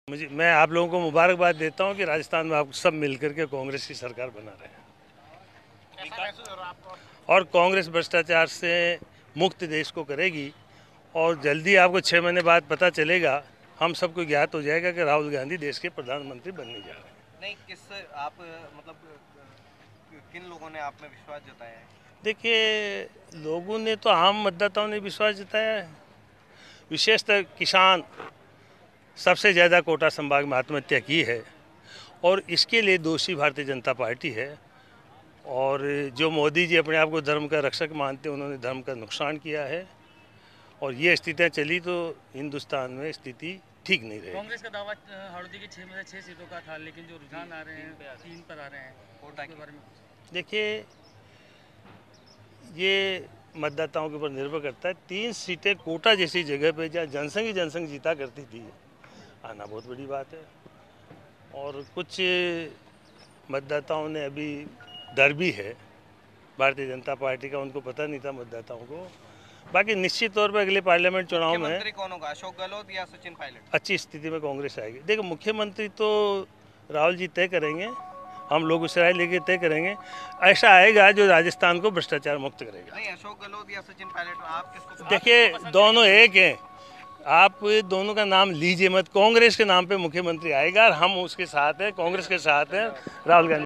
कौन बनेगा मुख्यमंत्री- गहलोत या पायलट
कोटा. जिले की 6 विधानसभा सीटों में तीन पर कांग्रेस और तीन पर भाजपा आगे चल रही है. पीपल्दा सीट से कांग्रेस उम्मीदवार रामनारायण मीणा अपने नजदीकी प्रतिद्वंदी भाजपा प्रत्याशी ममता शर्मा से 15 हजार से अधिक वोटों से जीते दर्ज किए हैं.